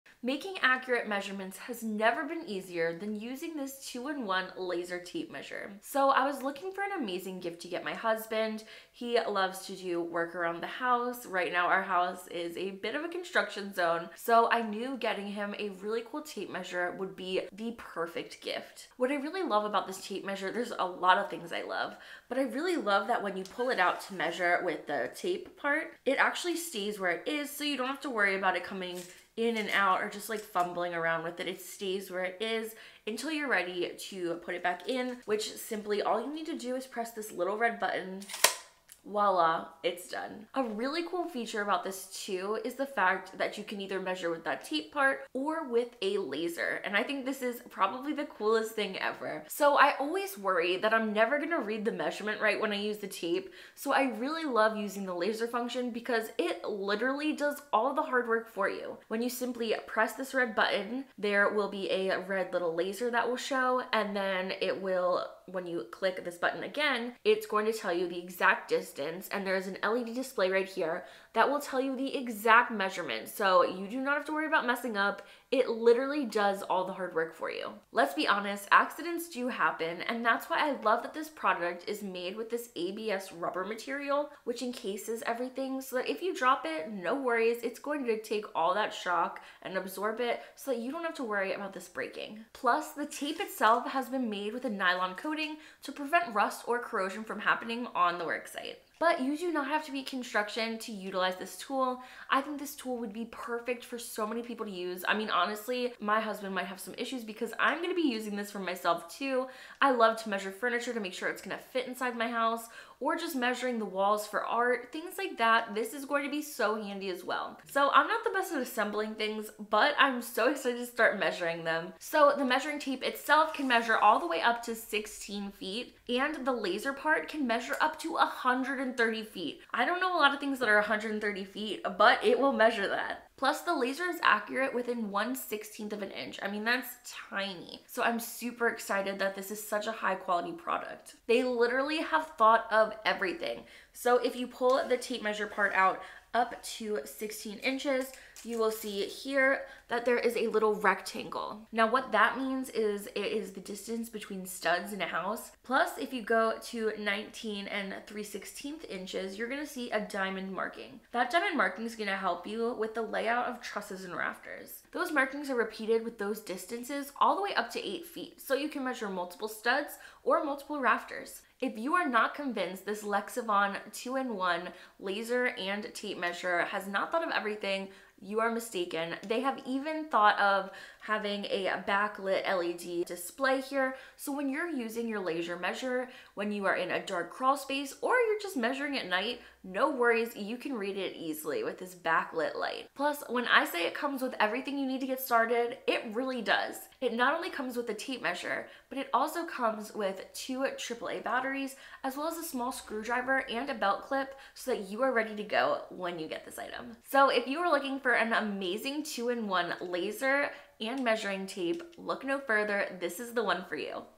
The Ultimate Gift for DIY Enthusiasts: Two-in-One Laser Tape Measure from Lexivon
Looking for the perfect gift for a DIY enthusiast? Look no further than the amazing two-in-one laser tape measure from Lexivon! This handy tool will make accurate measurements a breeze, whether you're working on a construction project or just doing some work around the house. The best part? The tape measure stays in place when you pull it out, eliminating any frustration. Get this ultimate gift for your DIY-loving husband and make his day!